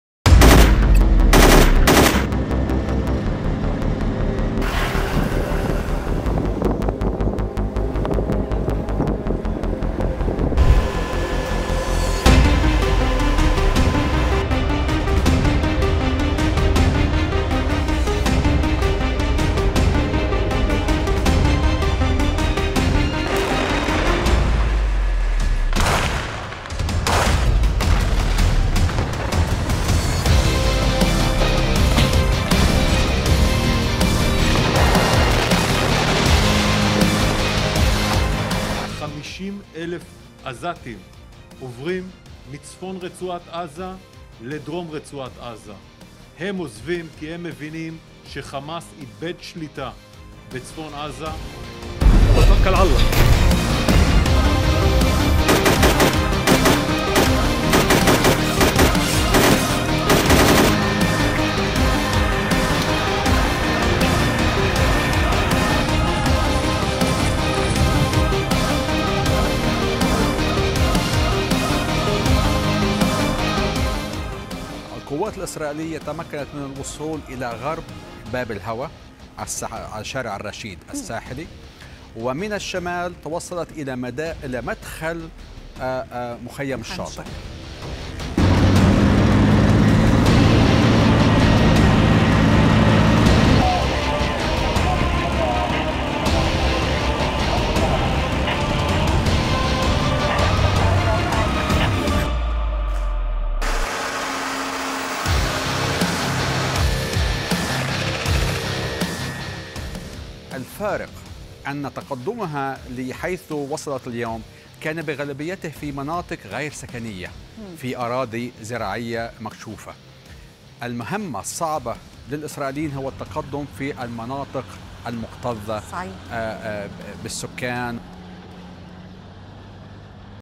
الوضع الميداني للمعارك بين حماس وإسرائيل بعد شهر من القتال
الوضع الميداني للمعارك بين حماس و إسرائيل بعد شهر من القتال.. خبراء عسكريون: "إسرائيل تقدمت بمناطق زراعية مفتوحة"
لمعرفة أحدث المستجدات.. عبر البث المباشر من هنا ⤵
وبإمكانكم متابعة الحدث على منصات التواصل